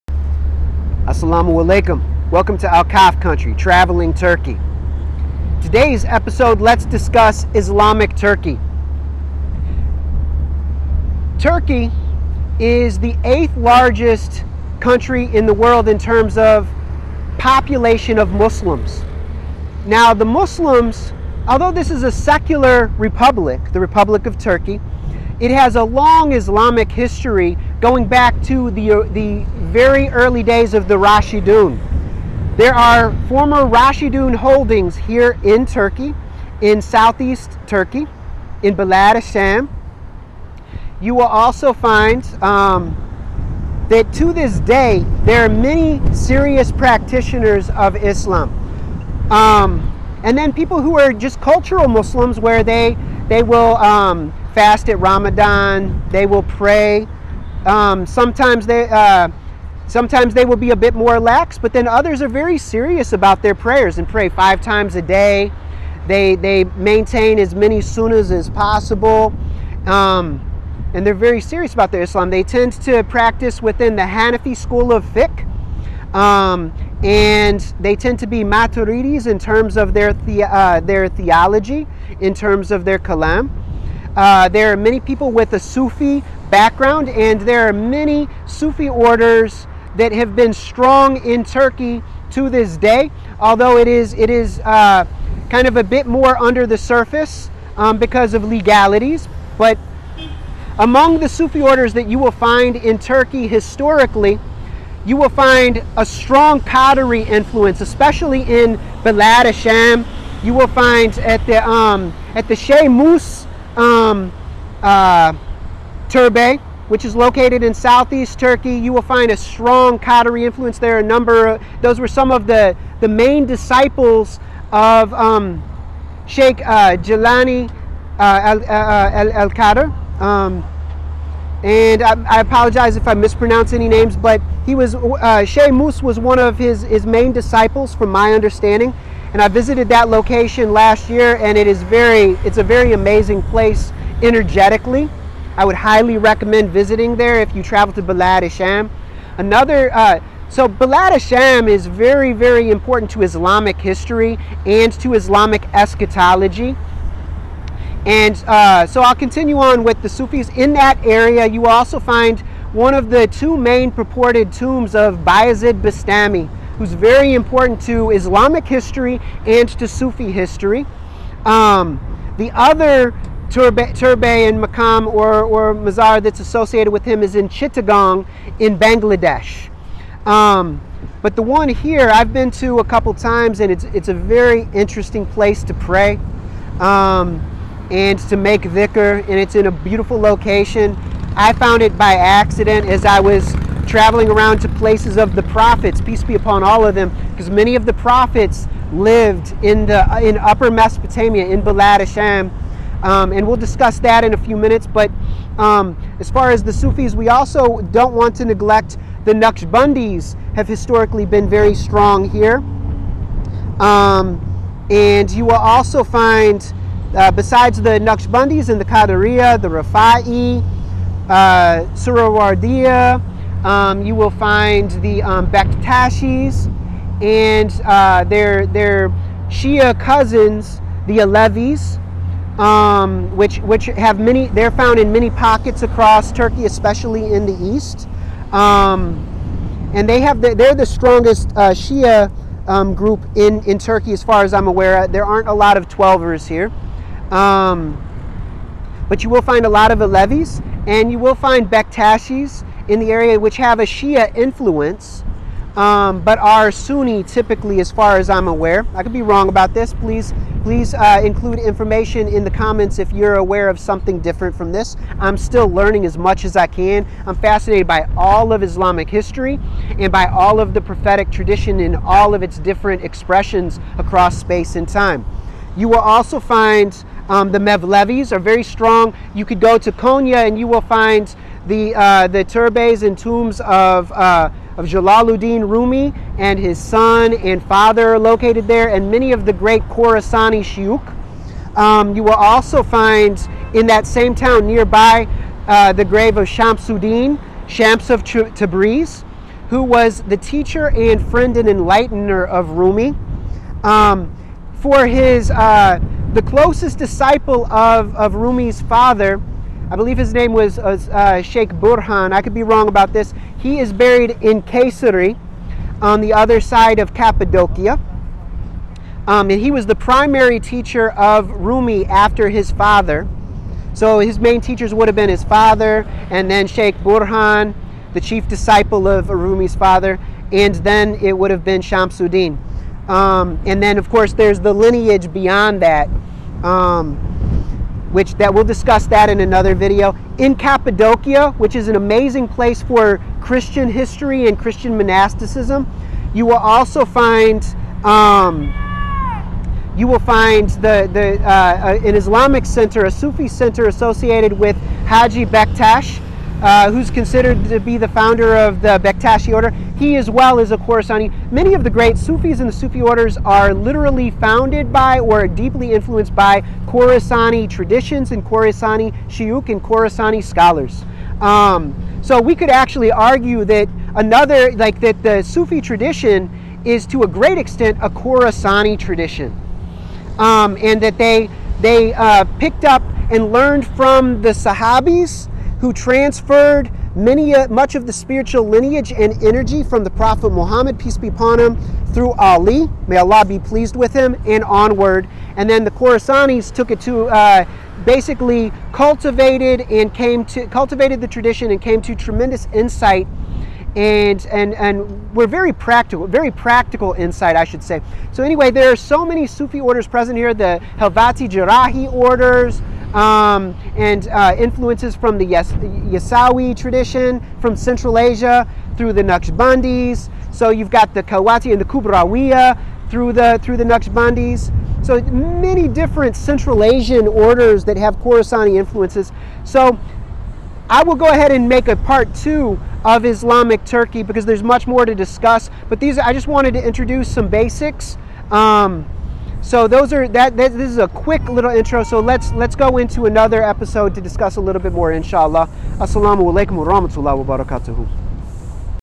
Traveling Islamic Turkey, Part 1 (Walking the Earth; Turkey)
The first video of a 2 part discussion about some amazing spiritual and historic sites across Turkey, associated with Islam and the broader prophetic tradition.